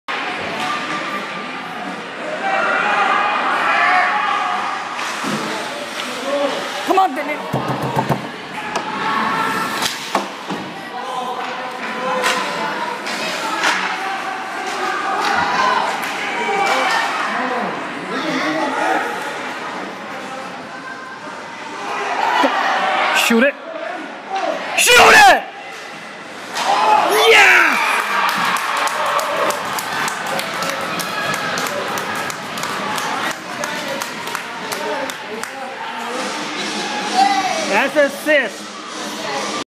If it did not work, try it again
They scored 2nd power play goal. Emma assist, Danielle goal.